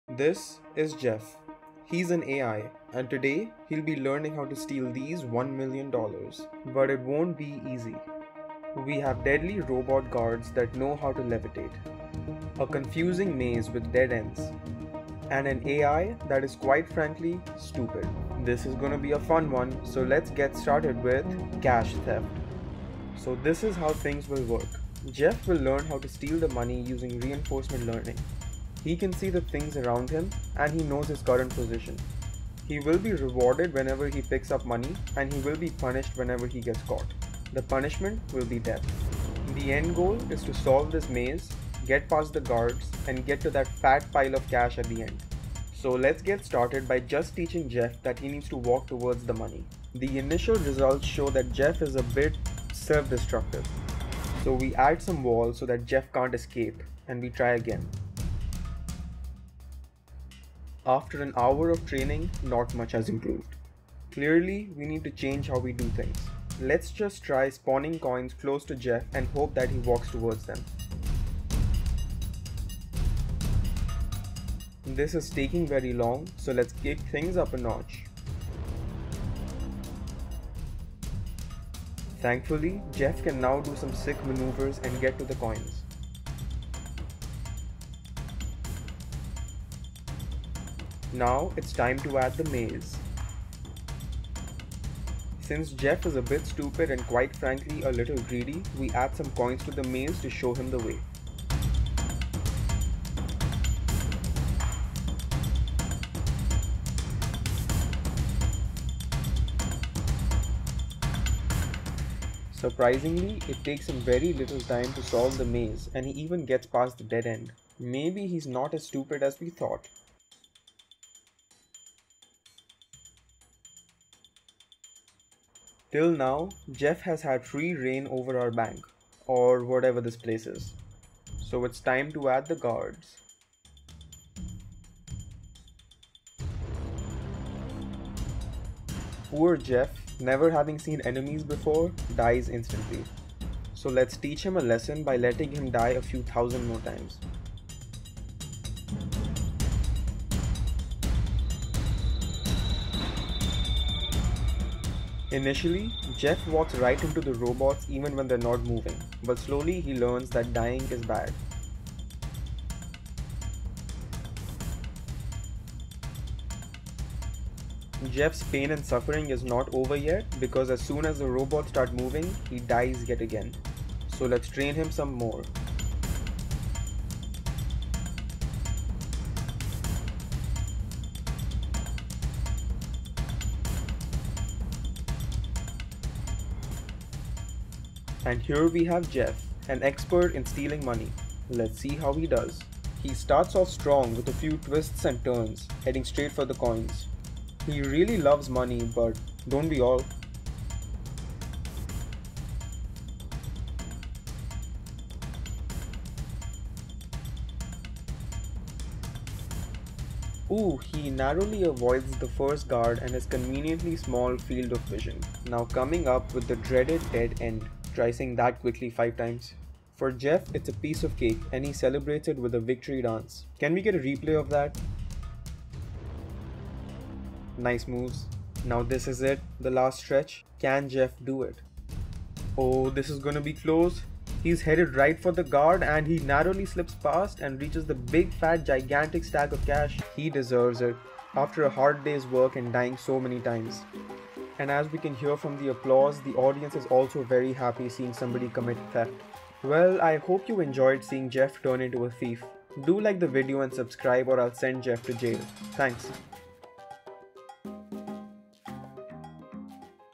A.I. Learns to Steal Using Reinforcement Learning
In this video, we will teach an AI how to steal money. We will be having some fun with Unity ML agents and reinforcement learning in this one as our AI Jeff learns how to navigate mazes, pick up coins, and get past deadly robots. Any resemblance to Money Heist is purely coincidental :)

Thank you lord MacLeod for your sick beats